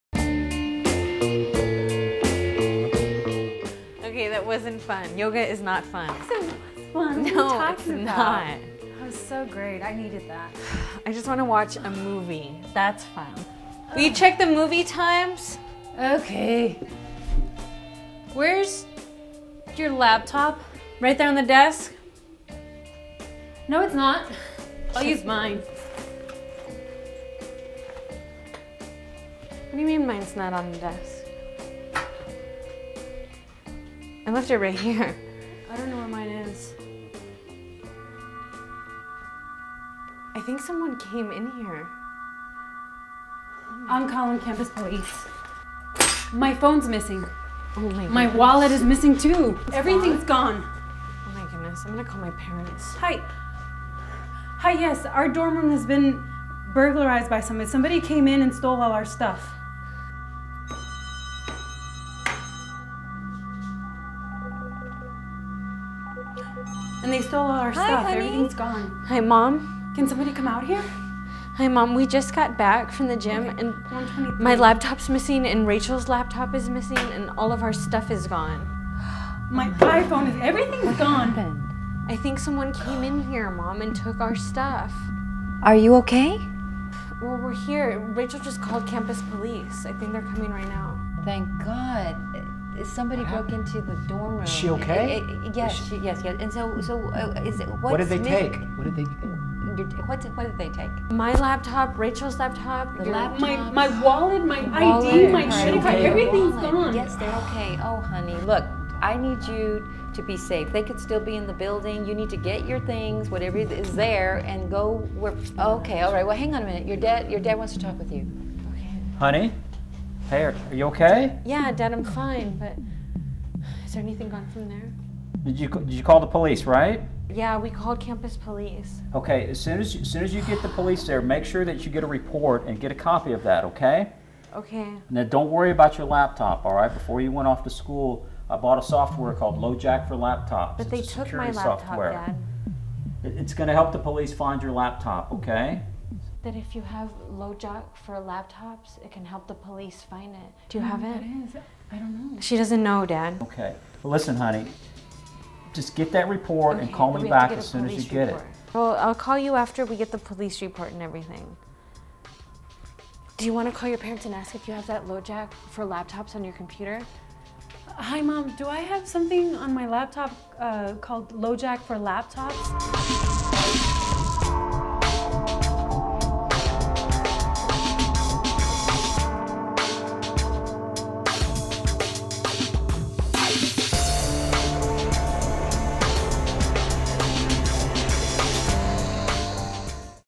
the dorm room trailer - lojack for laptops
what happens when your laptop gets stolen from your dorm room?